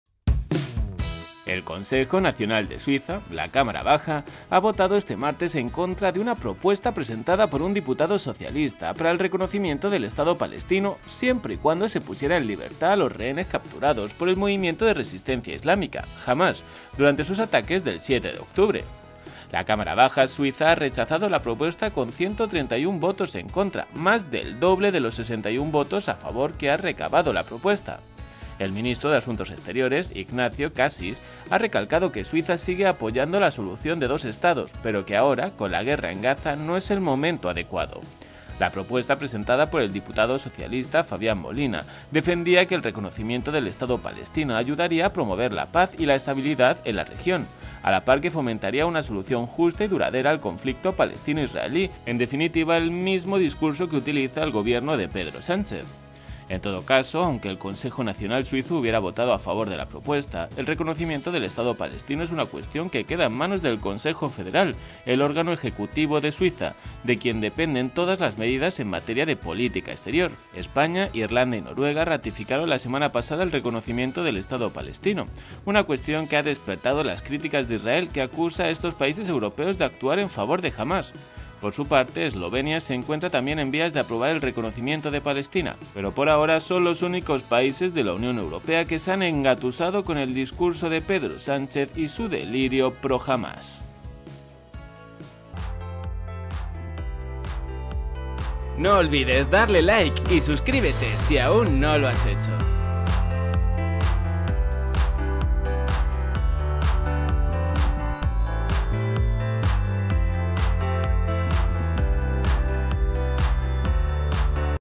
Suiza HUMILLA a Pedro Sánchez y SE NIEGA a reconocer a Palestina
El Consejo Nacional de Suiza (Cámara Baja) ha votado este martes en contra de una propuesta presentada por un diputado socialista para el reconocimiento del Estado palestino siempre y cuando se pusiera en libertad a los rehenes capturados por el Movimiento de Resistencia Islámica (Hamás) durante sus ataques del 7 de octubre.

La Cámara Baja suiza ha rechazado la propuesta con 131 votos en contra, más del doble de los 61 votos a favor que ha recabado la propuesta. El ministro de Asuntos Exteriores, Ignazio Cassis, ha recalcado que Suiza sigue apoyando la solución de dos Estados pero que ahora, con la guerra en Gaza, no es el momento adecuado.

La propuesta presentada por el diputado socialista Fabian Molina defendía que el reconocimiento del Estado palestino ayudaría a promover la paz y la estabilidad en la región, a la par que fomentaría una solución justa y duradera al conflicto palestino-israelí, según recoge el portal de noticias SwissInfo.

En todo caso, aunque el Consejo Nacional suizo hubiera votado a favor de la propuesta, el reconocimiento del Estado palestino es una cuestión que queda en manos del Consejo Federal, el órgano ejecutivo de Suiza, de quien dependen todas las medidas en materia de política exterior.

España, Irlanda y Noruega ratificaron la semana pasada el reconocimiento del Estado palestino, una cuestión que ha despertado las críticas de Israel, que acusa a estos países europeos de actuar en favor de Hamás. Por su parte, Eslovenia se encuentra también en vías de aprobar el reconocimiento de Palestina.